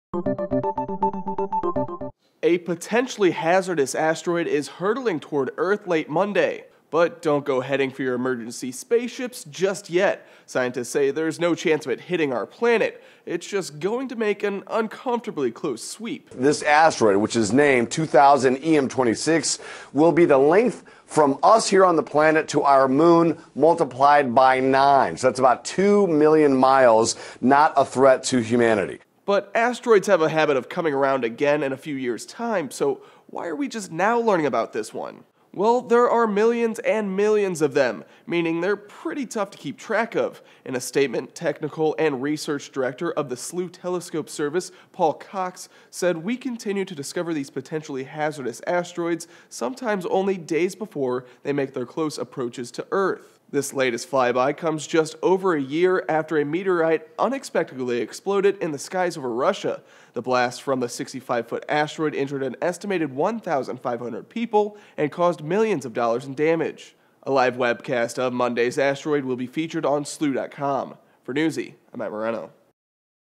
Huge Asteroid To Zip Past Earth Late Monday
Asteroid 2000 EM26 is nearly 900 feet in diameter and will make a close, but not dangerous, encounter with the earth.